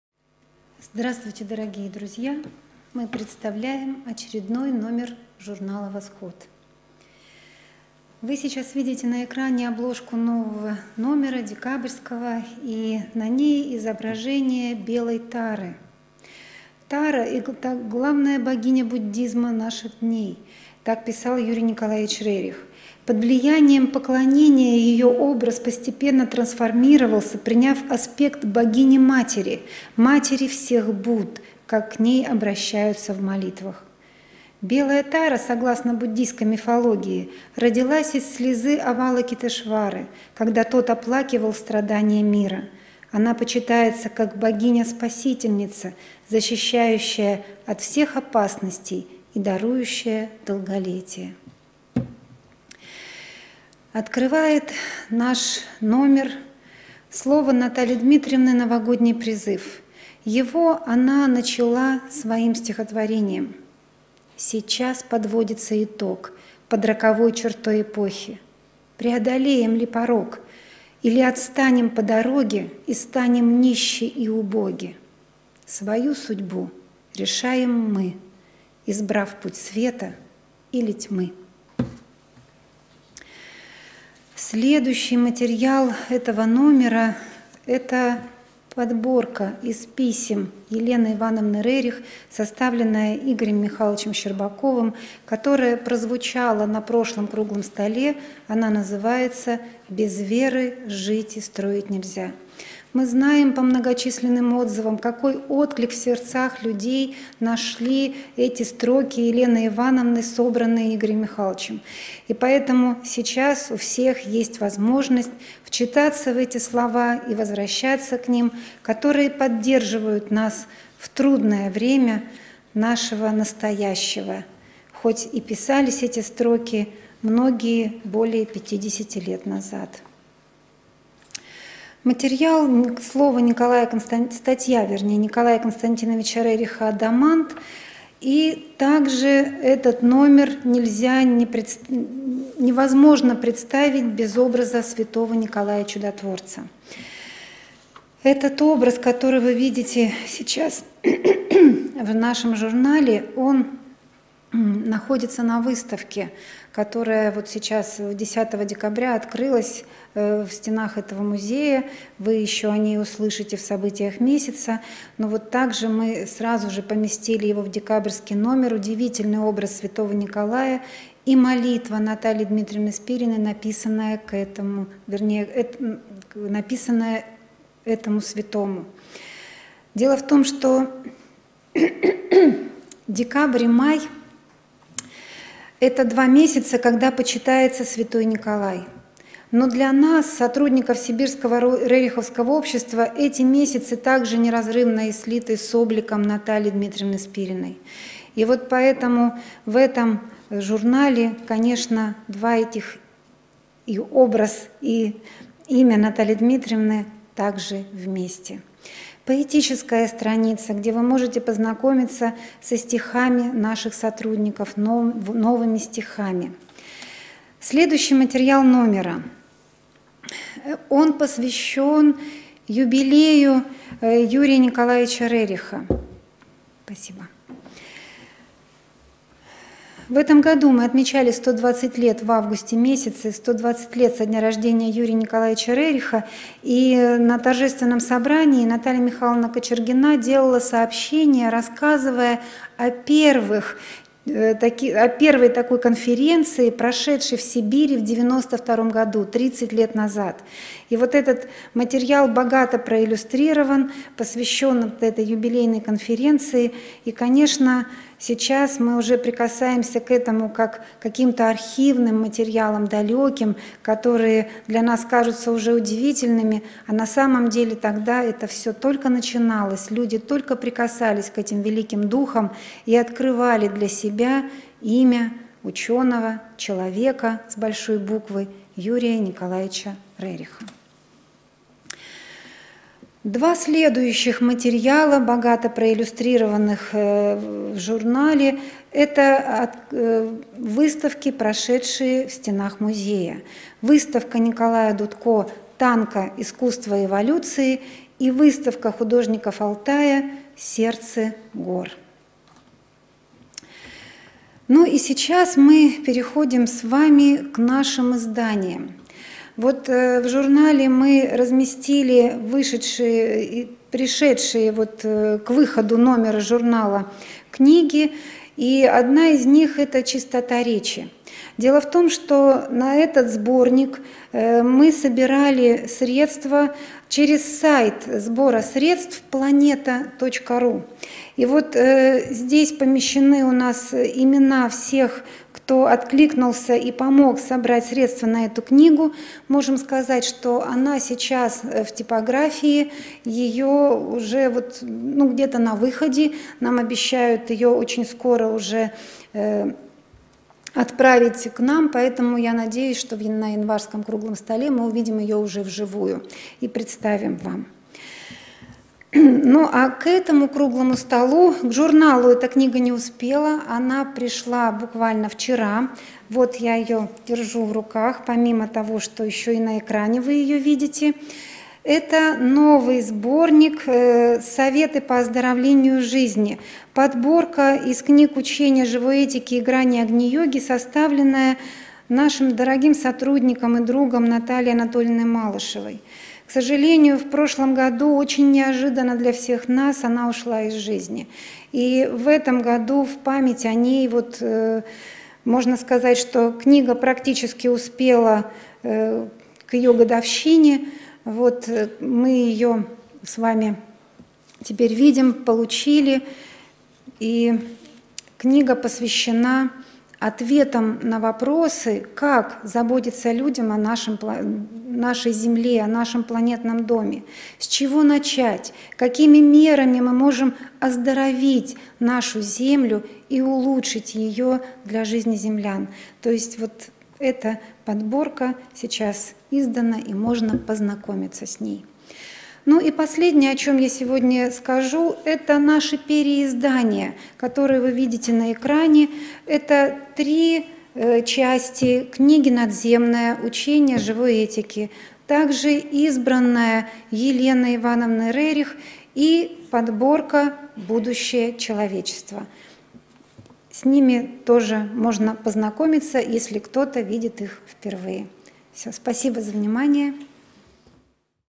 2022 Декабрь. Новости Издательского центра СибРО «Россазия»
④. Новости Издательского центра СибРО «Россазия» (Ю.В. Цыганкова).
[00:00] Журнал "Восход" № 12 (344), Декабрь, 2022
[05:10] Чистота речи / Сборник статей.
[06:11] "Советы по оздоровлению жизни". Составитель подборки Малышева Н. А.
[07:12] Наши переиздания:
   •  Учение Живой Этики (Агни Йога) / 13. НАДЗЕМНОЕ, 3 части. (Переиздание).
   •  Елена Ивановна Рерих / Избранное: фрагменты из писем.
   •  Будущее человечества / Из Граней Агни Йоги.

Благодарим наших постоянных помощников!